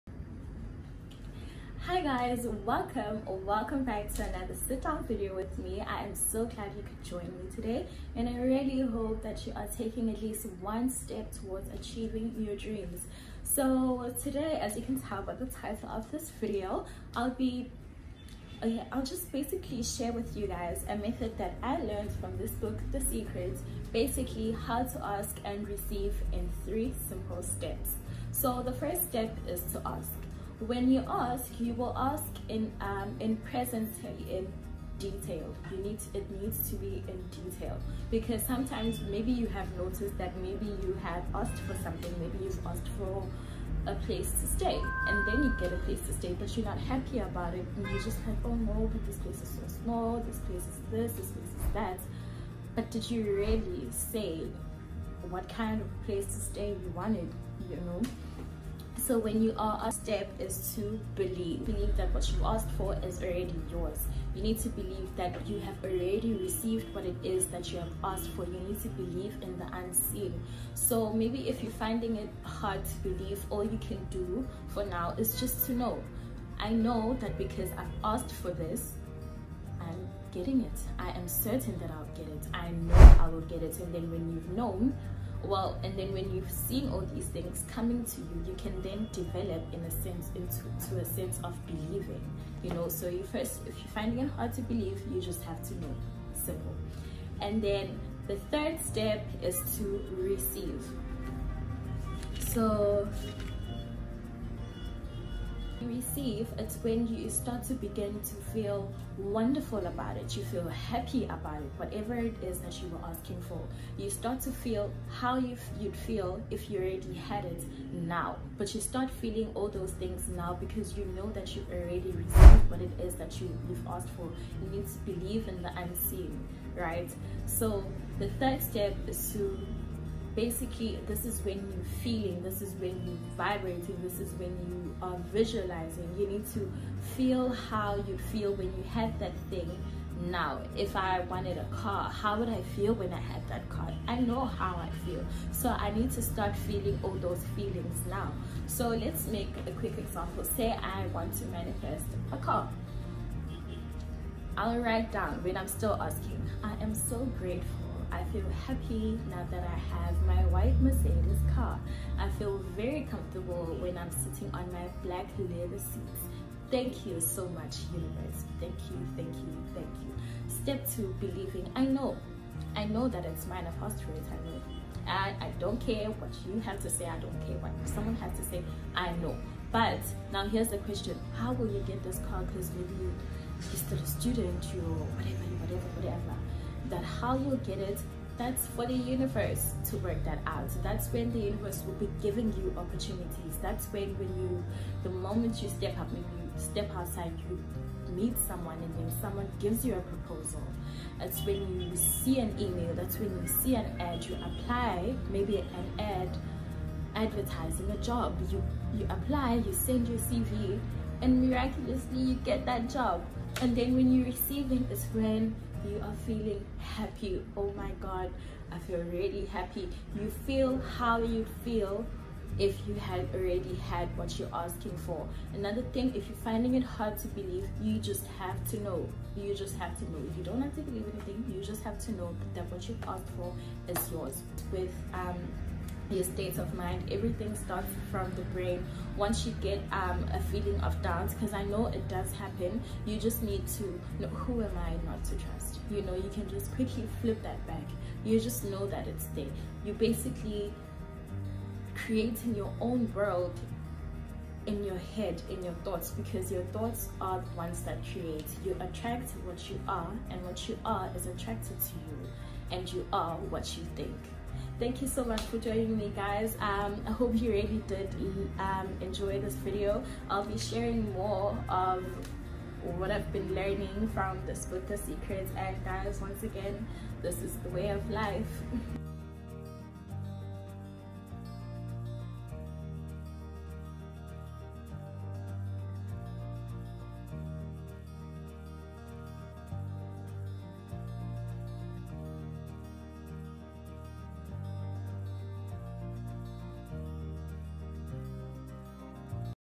ASK AND YOU SHALL RECEIVE IN JUST 3 STEPS 🤩
Hello Kind humans 😃

I am so glad you could join me again today. 🌸

Today I have 3 steps to consider when you are asking from the Universe to share with you guys that I got from the book “The Secret” by Rhonda Byrne.

I hope you guys enjoy and Stay blessed 😇